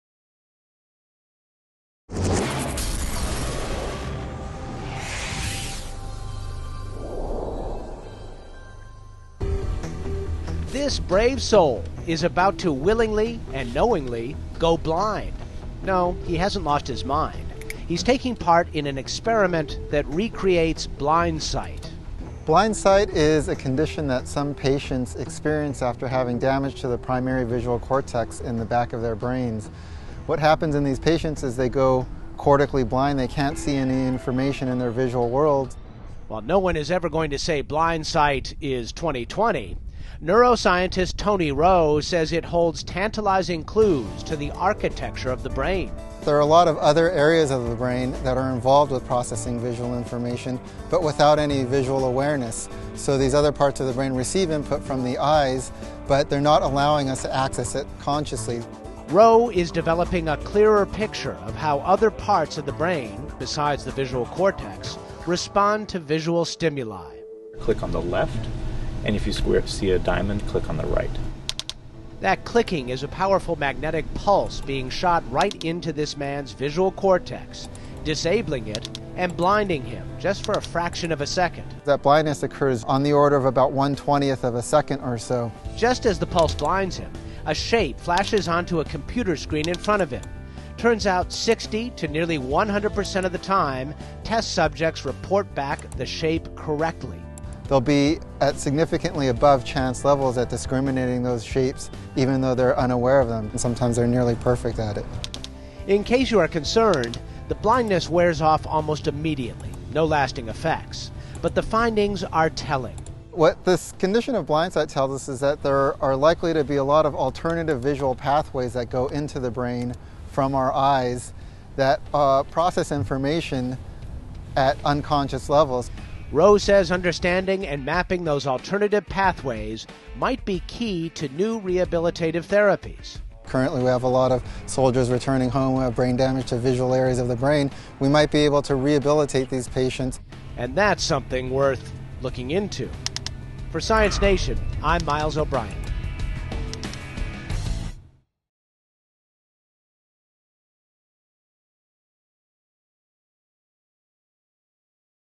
Seeing Beyond the Visual Cortex | Science Nation
It's a chilling thought - losing the sense of sight because of severe injury or damage to the brain's visual cortex. But, is it possible to train a damaged or injured brain to "see" again after such a catastrophic injury? Yes, according to Tony Ro, a neuroscientist at The City College of New York, who is artificially recreating a condition called Blindsight in his lab.  "Blindsight is a condition that some patients experience after having damage to the primary visual cortex in the back of their brains. What happens in these patients is they go cortically blind, yet they can still discriminate visual information, albeit without any awareness." explains Ro. While no one is ever going to say Blindsight is 20/20, Ro says it holds tantalizing clues to the architecture of the brain. With support from the National Science Foundation, Ro is developing a clearer picture of how other parts of the brain, besides the visual cortex, respond to visual stimuli. He says understanding and mapping those alternative pathways might be the key to new rehabilitative therapies.

or

or